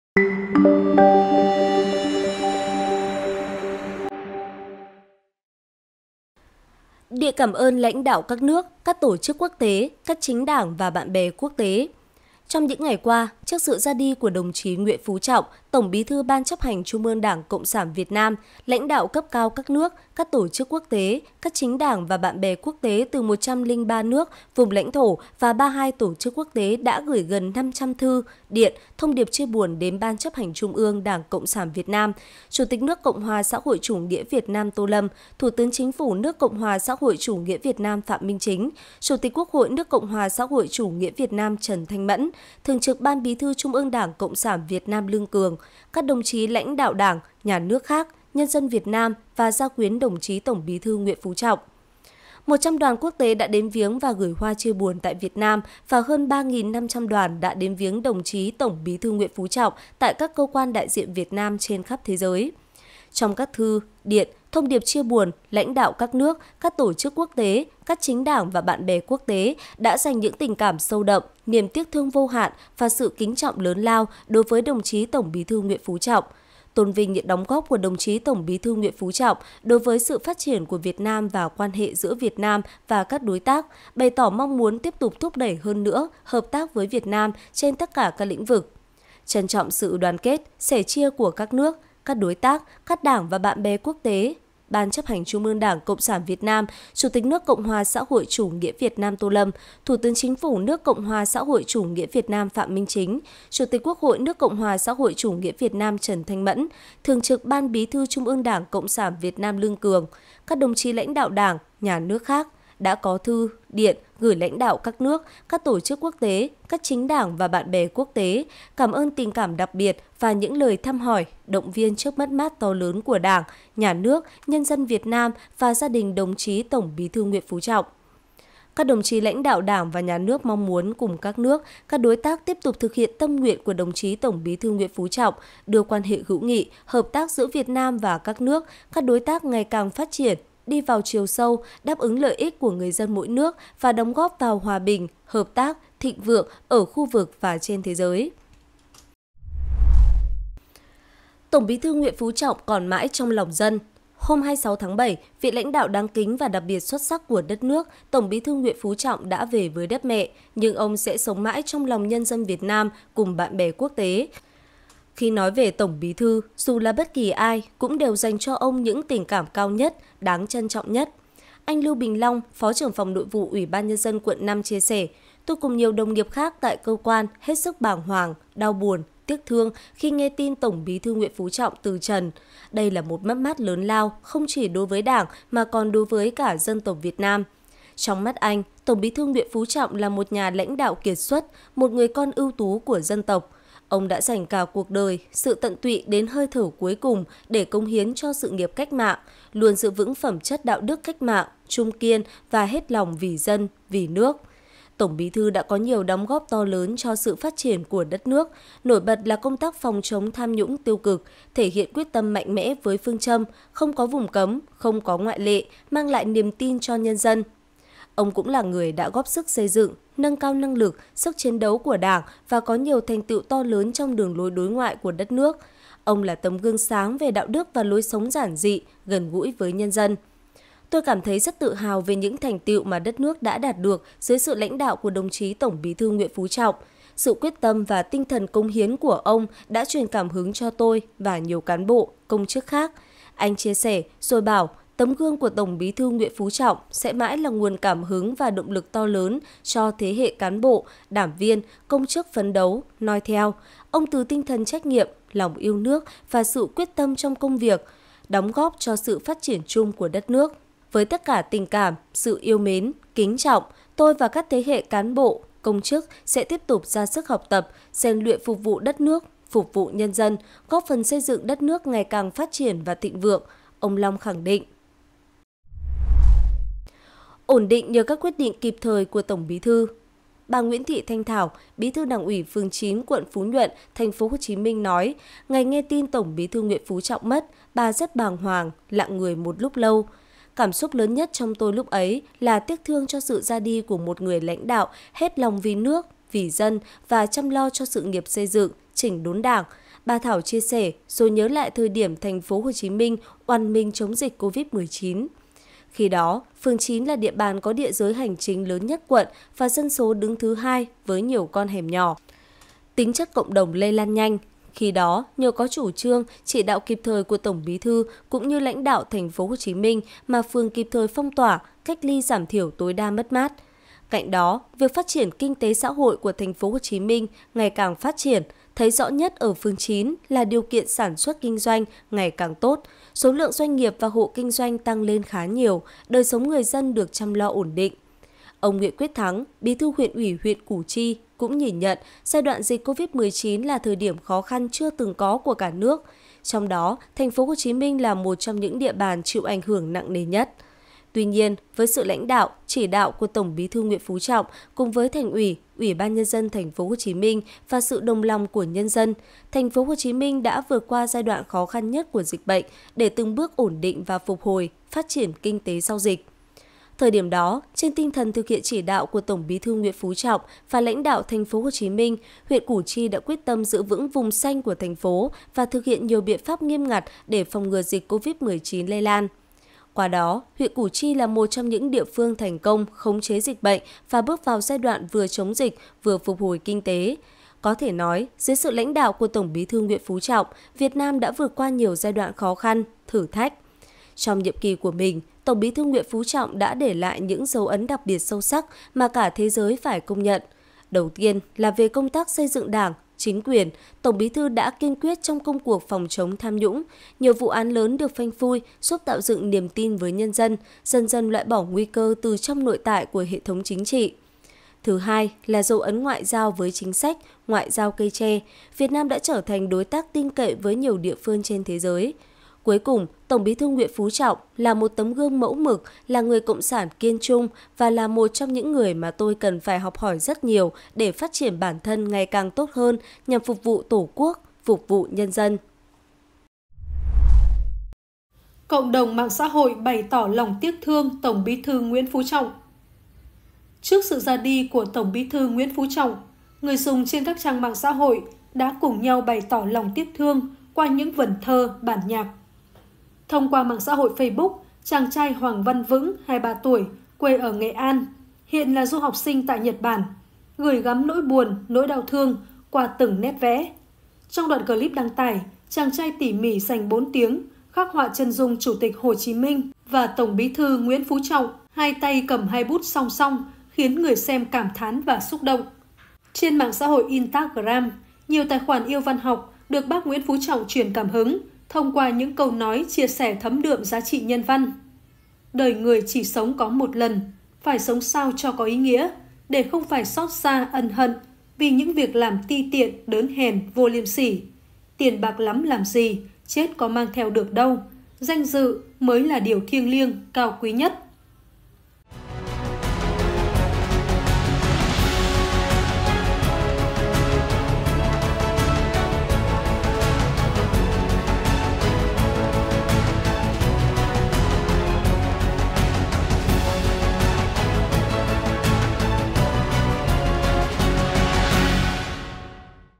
Đảng, Nhà nước gửi thư, điện cảm ơn các lãnh đạo, tổ chức, chính đảng và bạn bè quốc tế | Thời sự
PLO | Đảng, Nhà nước gửi thư, điện cảm ơn các lãnh đạo, tổ chức, chính đảng và bạn bè quốc tế
Trong những ngày qua, trước sự ra đi của Tổng Bí thư Nguyễn Phú Trọng, lãnh đạo cấp cao từ 103 nước, vùng lãnh thổ và 32 tổ chức quốc tế đã gửi gần 500 thư, điện, và thông điệp chia buồn đến Ban Chấp hành Trung ương Đảng Cộng sản Việt Nam và các lãnh đạo cấp cao của Việt Nam. Đồng thời, 100 đoàn quốc tế đã đến viếng và gửi hoa chia buồn tại Việt Nam, cùng hơn 3.500 đoàn viếng tại các cơ quan đại diện Việt Nam trên toàn thế giới.
Trong các thư, điện, và thông điệp chia buồn, lãnh đạo các nước và tổ chức quốc tế đã bày tỏ tình cảm sâu đậm, niềm tiếc thương vô hạn và sự kính trọng đối với Tổng Bí thư Nguyễn Phú Trọng. Họ tôn vinh những đóng góp của ông đối với sự phát triển của Việt Nam và quan hệ giữa Việt Nam và các đối tác quốc tế, đồng thời bày tỏ mong muốn tiếp tục thúc đẩy hợp tác với Việt Nam trên mọi lĩnh vực.
Ban Chấp hành Trung ương Đảng Cộng sản Việt Nam, Chủ tịch nước Tô Lâm, Thủ tướng Phạm Minh Chính, Chủ tịch Quốc hội Trần Thanh Mẫn, Thường trực Ban Bí thư Lương Cường, cùng các lãnh đạo Đảng và Nhà nước khác đã gửi thư, điện cảm ơn đến lãnh đạo các nước, các tổ chức quốc tế, các chính đảng và bạn bè quốc tế vì tình cảm và lời thăm hỏi, động viên trước mất mát to lớn của Đảng, Nhà nước, Nhân dân Việt Nam và gia đình Tổng Bí thư Nguyễn Phú Trọng.
Các lãnh đạo Đảng và Nhà nước Việt Nam mong muốn cùng các đối tác quốc tế thực hiện tâm nguyện của Tổng Bí thư Nguyễn Phú Trọng, đưa quan hệ hữu nghị và hợp tác giữa Việt Nam và các nước ngày càng phát triển, đóng góp vào hòa bình, hợp tác và thịnh vượng khu vực và toàn cầu.